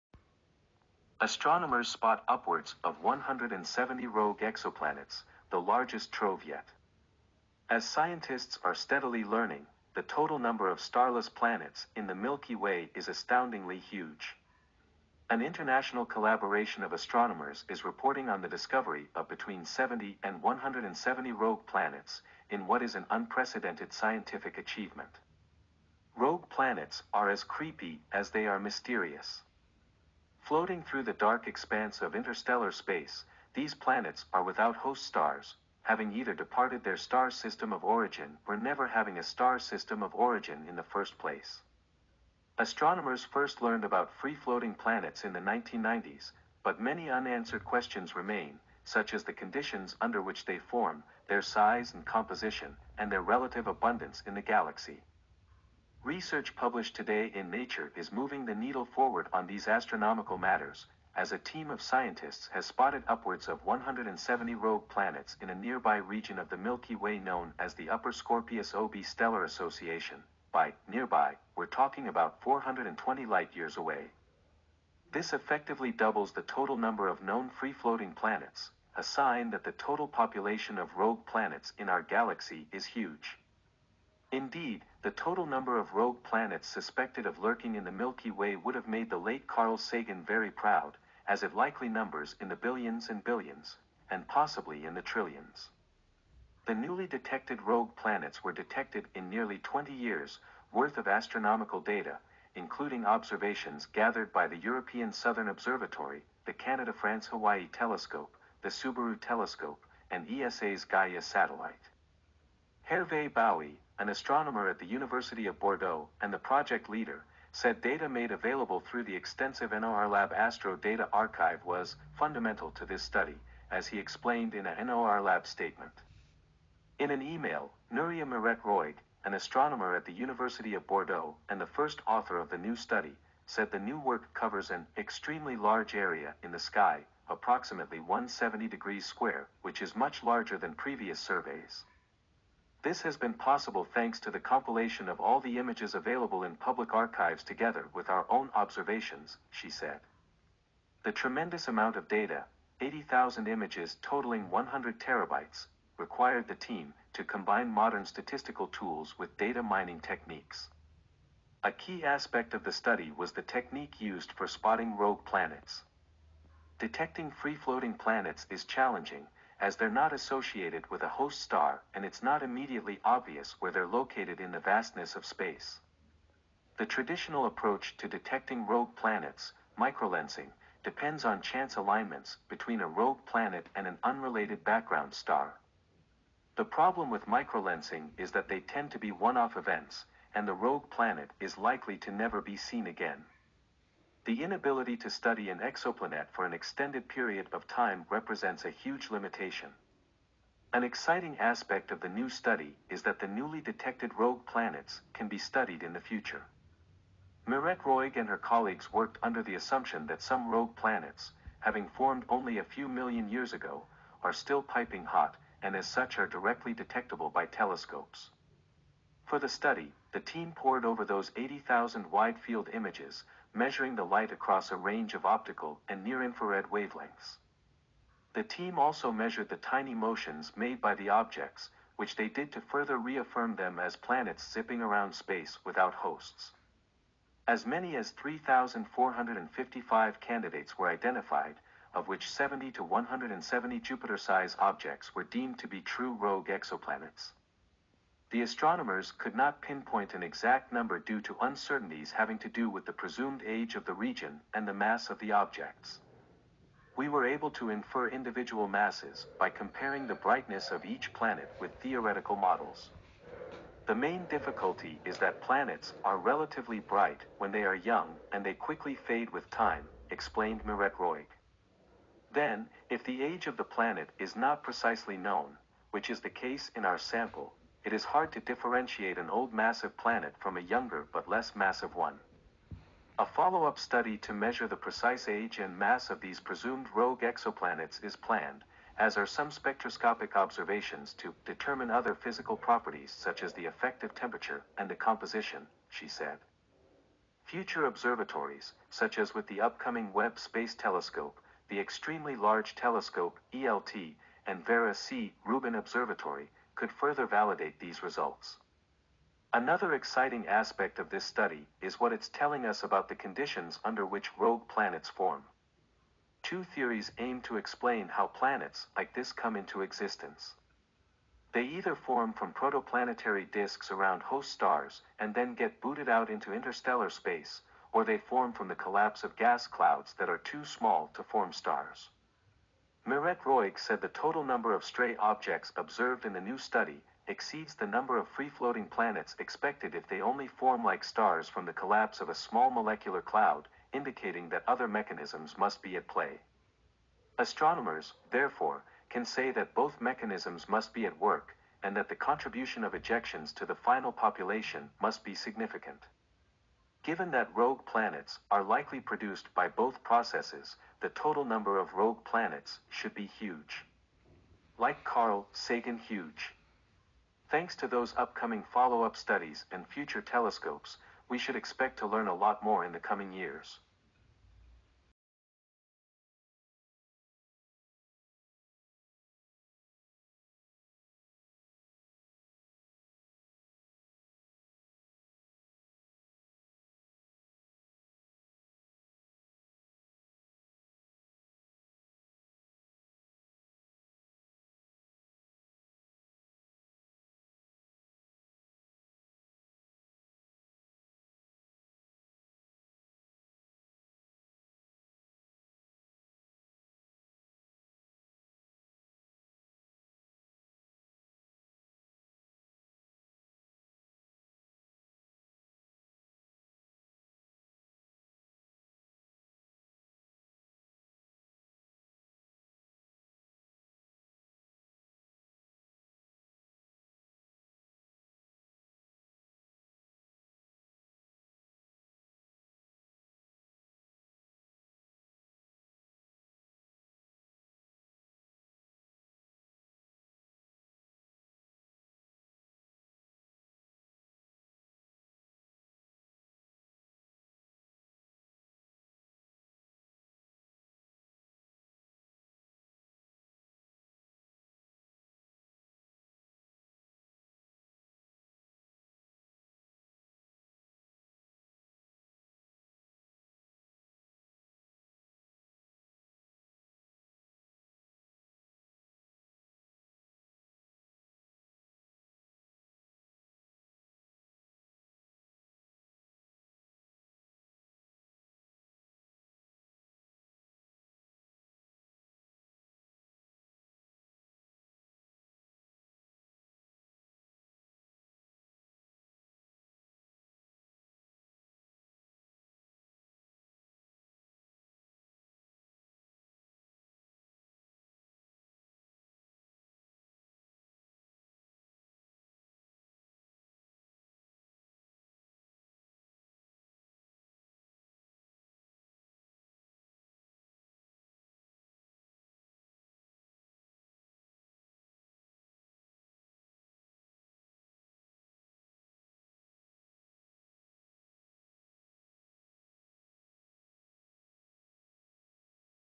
Astronomers Spot Upwards of 170 Rogue Exoplanets, the Largest Trove Yet
As scientists are steadily learning, the total number of starless planets in the Milky Way is astoundingly huge.
An international collaboration of astronomers is reporting on the discovery of between 70 and 170 rogue planets, in what is an unprecedented scientific achievement.

Rogue planets are as creepy as they are mysterious. Floating through the dark expanse of interstellar space, these planets are without host stars, having either departed their star system of origin or never having a star system of origin in the first place. Astronomers first learned about free-floating planets in the 1990s, but many unanswered questions remain, such as the conditions under which they form, their size and composition, and their relative abundance in the galaxy.

Research published today in Nature is moving the needle forward on these astronomical matters, as a team of scientists has spotted upwards of 170 rogue planets in a nearby region of the Milky Way known as the Upper Scorpius OB stellar association (by “nearby” we’re talking about 420 light years away). This effectively doubles the total number of known free-floating planets—a sign that the total population of rogue planets in our galaxy is huge. Indeed, the total number of rogue planets suspected of lurking in the Milky Way would’ve made the late Carl Sagan very proud, as it likely numbers in the billions and billions—and possibly in the trillions.

The newly detected rogue planets were detected in nearly 20 years’ worth of astronomical data, including observations gathered by the European Southern Observatory, the Canada-France-Hawaii Telescope, the Subaru Telescope, and ESA’s Gaia satellite. Hervé Bouy, an astronomer at the University of Bordeaux and the project leader, said data made available through the extensive NOIRLab Astro Data Archive was “fundamental to this study,” as he explained in a NOIRLab statement.
In an email, Núria Miret-Roig, an astronomer at the University of Bordeaux and the first author of the new study, said the new work covers an “extremely large area in the sky,” approximately 170 degrees square, which is much larger than previous surveys. “This has been possible thanks to the compilation of all the images available in public archives together with our own observations,” she said. The tremendous amount of data—80,000 images totaling 100 TB—required the team “to combine modern statistical tools with data mining techniques.”

A key aspect of the study was the technique used for spotting rogue planets. Detecting free-floating planets is challenging, as they’re not associated with a host star and it’s not immediately obvious where they’re located in the vastness of space. The traditional approach to detecting rogue planets, microlensing, depends on chance alignments between a rogue planet and an unrelated background star. The problem with microlensing is that they tend to be one-off events, and the rogue planet is likely to never be seen again. The inability to study an exoplanet for an extended period of time represents a huge limitation.

An exciting aspect of the new study is that the newly detected rogue planets can be studied in the future. Miret-Roig and her colleagues worked under the assumption that some rogue planets, having formed only a few million years ago, are still piping hot, and as such are directly detectable by telescopes. For the study, the team pored over those 80,000 wide-field images, measuring the light across a range of optical and near-infrared wavelengths. The team also measured the tiny motions made by the objects, which they did to further re-affirm them as planets zipping around space without hosts.

As many as 3,455 candidates were identified, of which 70 to 170 Jupiter-size objects were deemed to be true rogue exoplanets. The astronomers could not pinpoint an exact number due to uncertainties having to do with the presumed age of the region and the mass of the objects.

“We were able to infer individual masses by comparing the brightness of each planet with theoretical models. The main difficulty is that planets are relatively bright when they are young and they quickly fade with time,” explained Miret-Roig. “Then, if the age of the planet is not precisely known—which is the case in our sample—it is hard to differentiate an old massive planet from a younger but less massive one.”